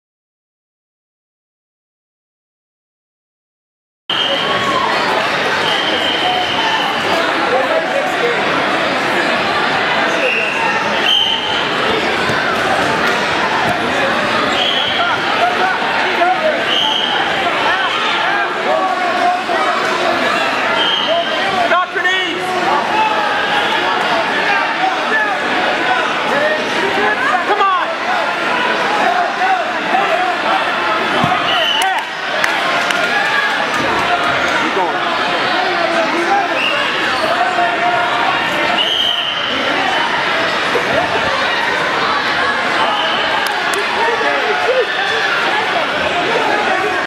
Carsten Match 1 Delaware 1_18_15
Carsten Cribbs wrestled in the Russ Hellickson league tournament at Delaware Hayes HS in Delaware, OH. He got the pin in ~30 seconds and went three for three on the day, all three were pins. He is 7 years old and weighs 50 pounds.